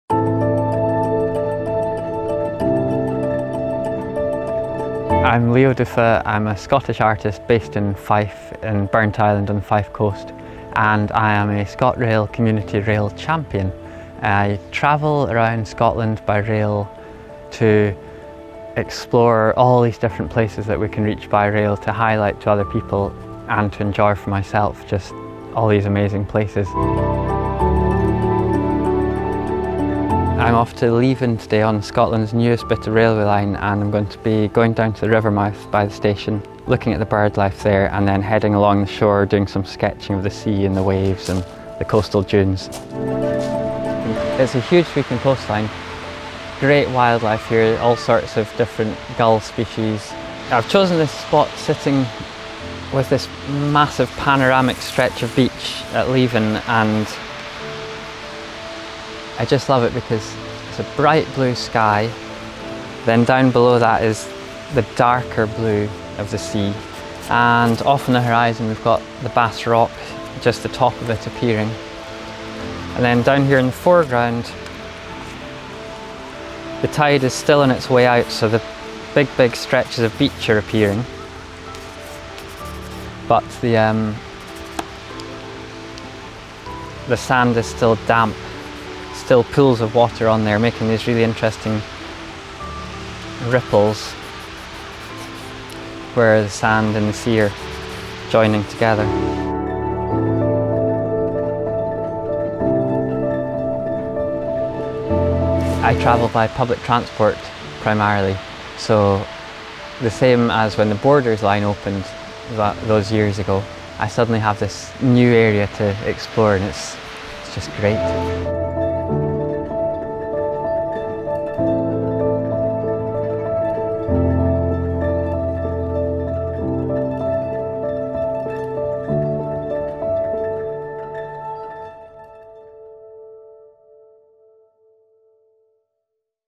Leo du Feu, Scottish artist and ScotRail Community Champion at Leven Beach, Fife
Meet Leo, Scottish artist & ScotRail Community Champion.

Travelling around Scotland by rail, he uses trains to explore new places, before capturing some local scenery through his art work.

Visiting Leven Beach, Leo shares how the scenery inspires him.